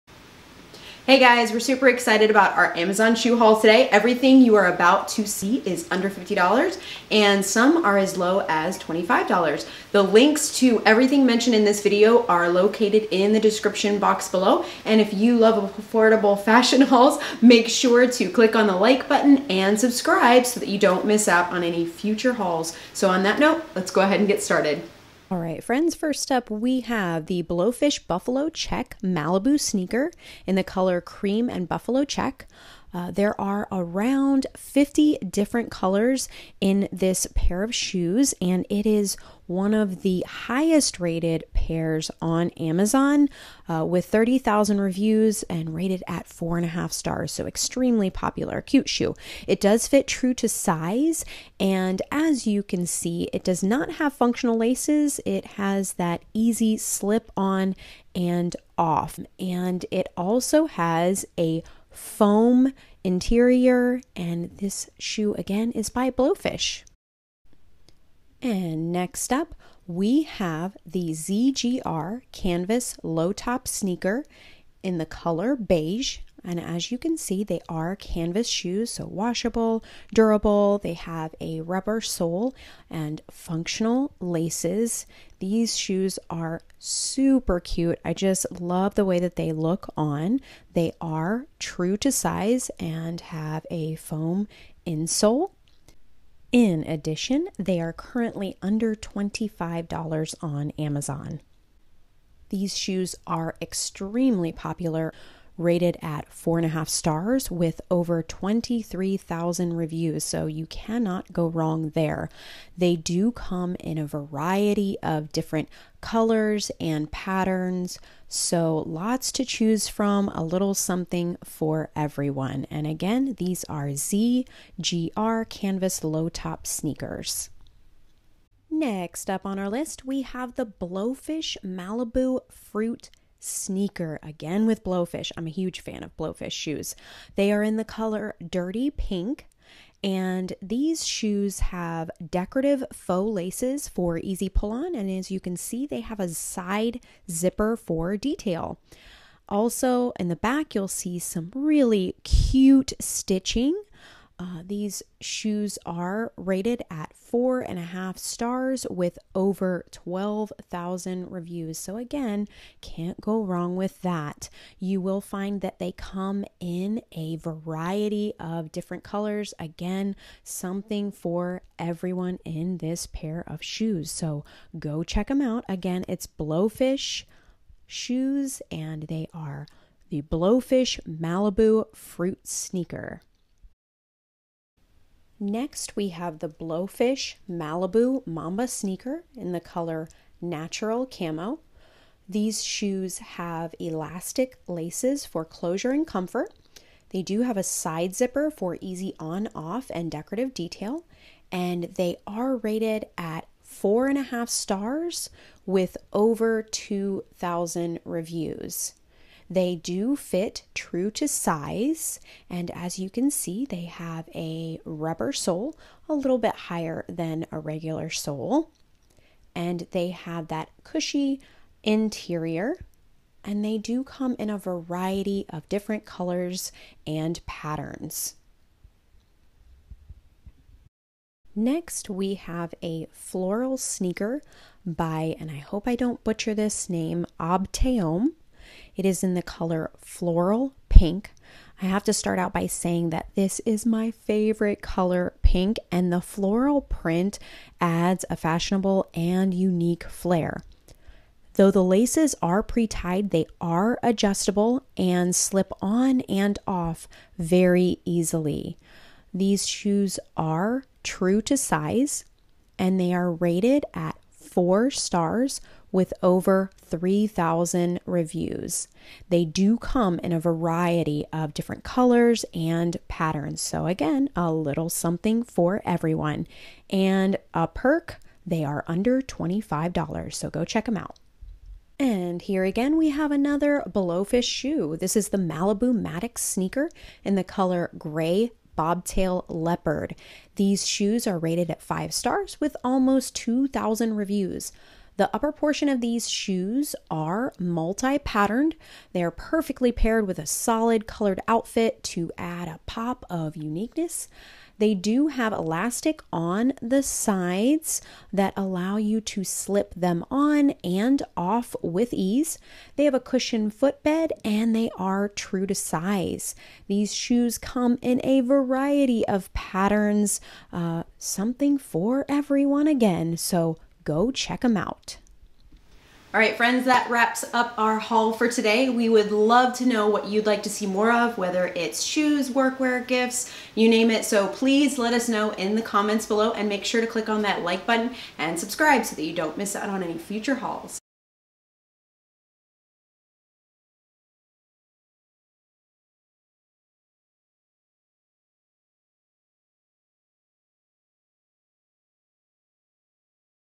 Amazon Sneakers Shoe Haul 2022
All under $50

Blowfish Buffalo Check Malibu Sneaker (Color- Cream Buffalo)

ZGR Canvas Low Top Sneaker (Color- Beige)

Blowfish Malibu Fruit Sneaker (Color- Dirty Pink)

Blowfish Malibu Mamba Sneaker (Color- Natural Camo)

Obtaom Floral Sneaker (Color- Floral Pink)

Blowfish Malibu Maddox Sneaker (Color- Grey Bobtail Leopard)

Outfits
ECOWISH Bomber Jacket (Color- Army Green)
Butt I Love You Jeans (Color- Dark Denim)
Striped T-shirt Wrap Dress (Color- Blue and white)
Denim Jean Jacket
Denim Jacket (not shown but love!)